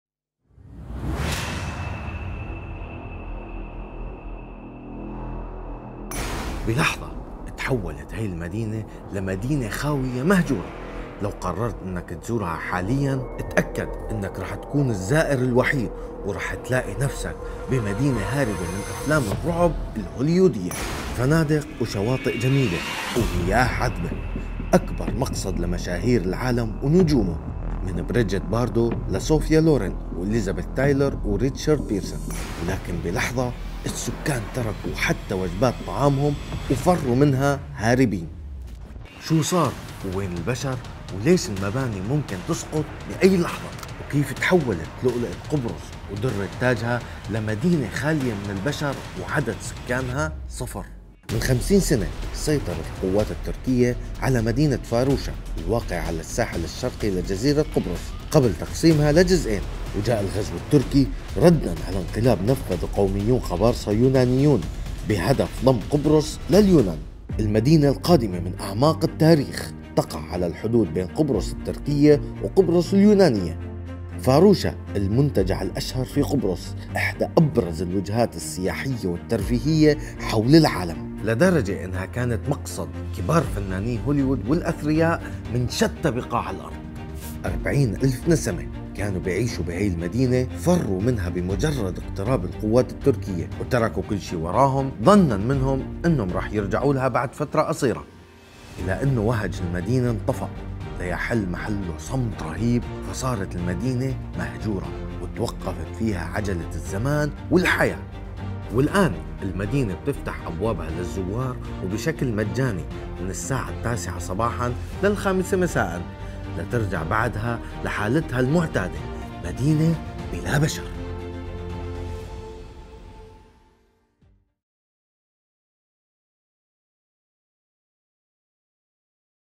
“⁠فاروشا”.. مدينة الأشباح التي توقف فيها الزمن
بلحظة تحولت هي المدينة إلى مدينة خاوية مهجورة
لو قررت تزورها حالياً
اتأكد أنك رح تكون الزائر الوحيد
وتلاقي مدينة هاربة من أفلام الرعب الهوليودية

فنادق وشواطئ جميلة ومياه عذبة
أكبر مقصد لمشاهير العالم ونجومه
من بريجيت باردو إلى صوفيا لورين وإليزابيث تايلور و ريتشارد بيرسون
ولكن بلحظة.. السكان تركوا حتى وجبات طعامهم وفروا منها هاربين!

شو صار ؟ وين البشر؟ وليش المباني ممكن تسقط بأي لحظة
وكيف تحولت لؤلؤة قبرص ودرة تاجها
إلى مدينة خاوية خالية من البشر وعدد سكانها صفر

من خمسين سنة سيطرت القوات التركية
على مدينة فاروشا الواقعة على الساحل الشرقي
لجزيرة قبرص قبل تقسيمها إلى جزأين
وجاء الغزو التركي ردا على انقلاب نفّذه قوميون
قبارصة يونانيون بهدف ضم قبرص الى اليونان.